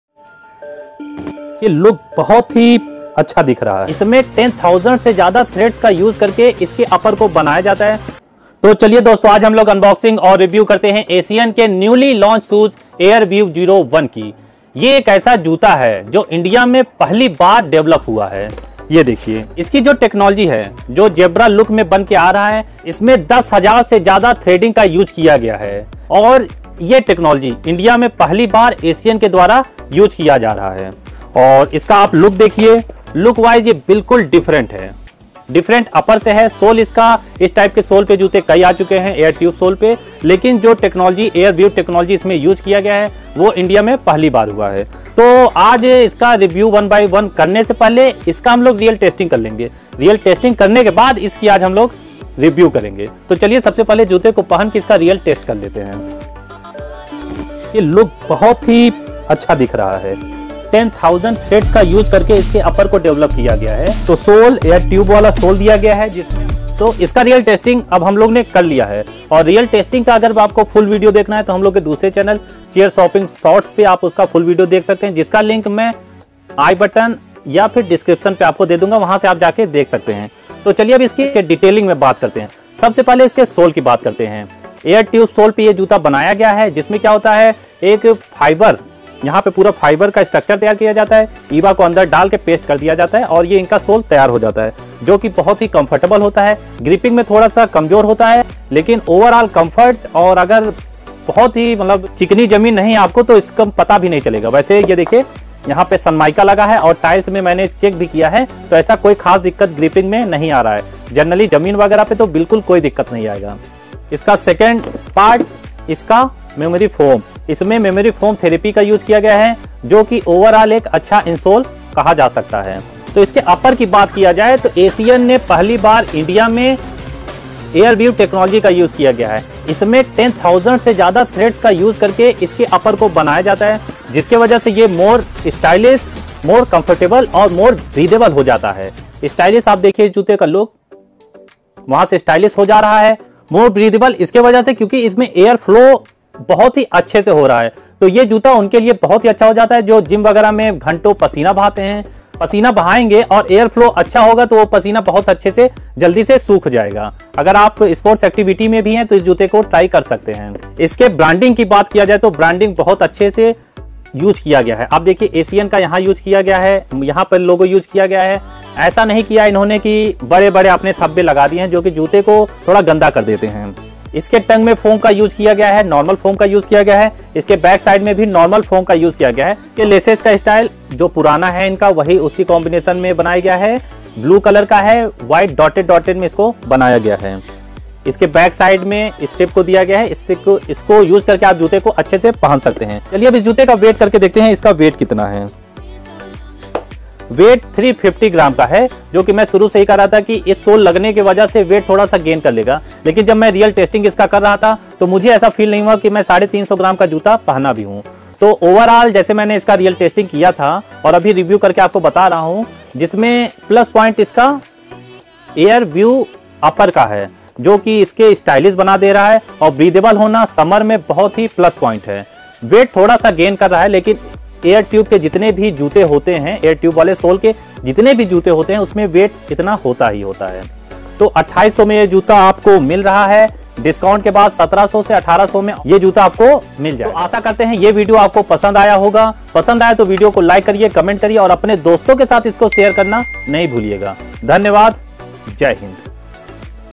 Unboxing and Review - Asian Airweave 01 sports shoes for men - Sports shoes under 2000
Video Description -

Welcome to our latest video, "Asian Sports Shoes Airweave 01"!

In this video, we will be introducing you to the latest addition to the Asian Sports Shoes family, the Airweave 01. This shoe has been designed with the latest technology and features to provide you with the ultimate sports performance and comfort.

Our expert team of designers and engineers have worked tirelessly to create a shoe that is not only stylish and fashionable, but also functional and practical. The Airweave 01 features a lightweight and breathable mesh upper, providing maximum ventilation for your feet during intense workouts.

The shoe also boasts a unique metacushion midsole, which provides superior cushioning and support to your feet, reducing the impact of each step and ensuring a comfortable ride. The outsole is made of durable rubber, providing excellent grip and traction on any surface.

Whether you're a professional athlete or a fitness enthusiast, the Asian Sports Shoes Airweave 01 is the perfect choice for your next sports shoe. So sit back, relax, and watch as we take you through the features and benefits of this amazing shoe.

Our video weapons :-
Samsung Mobile
Dell Laptop